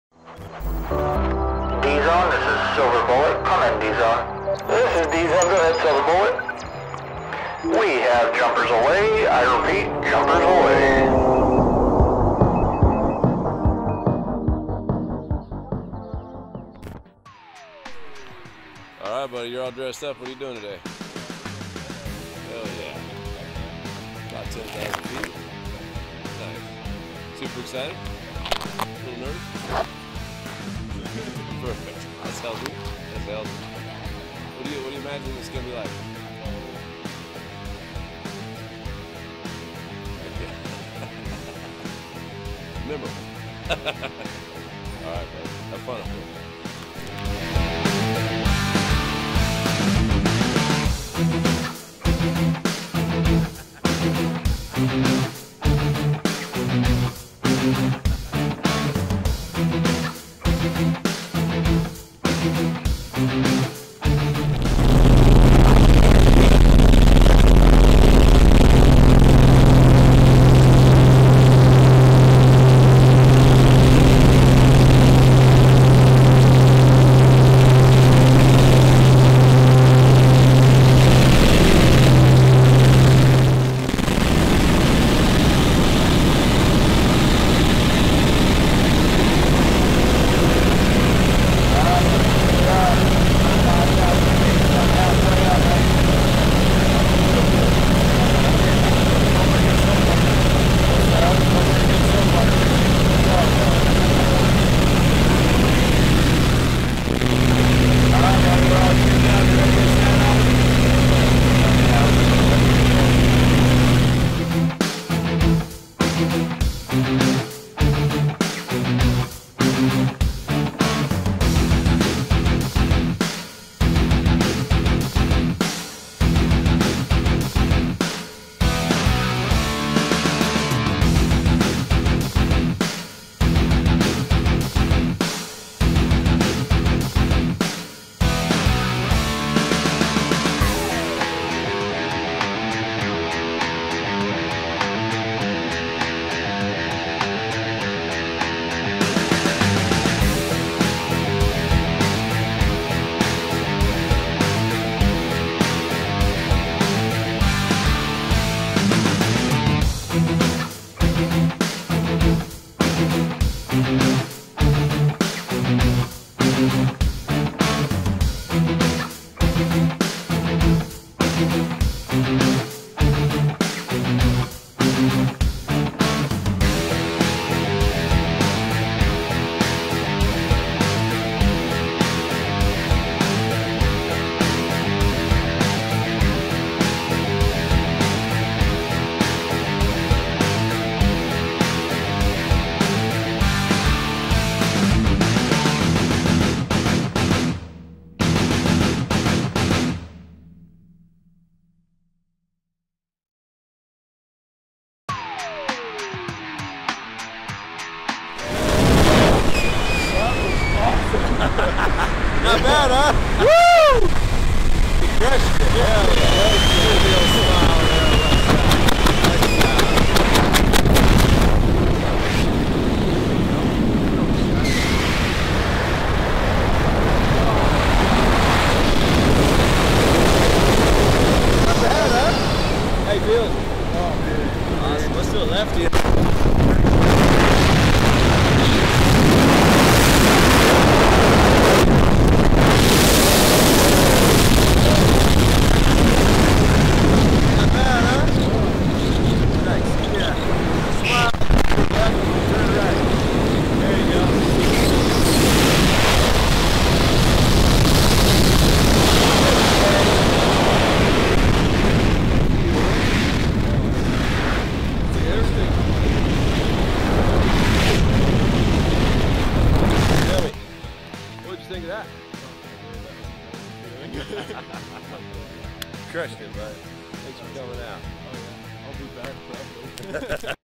Clinton Harman's Tandem skydive!
Clinton Harman's tandem skydive on Sunday, August 1, 2021 with Stanley DJ Higgins.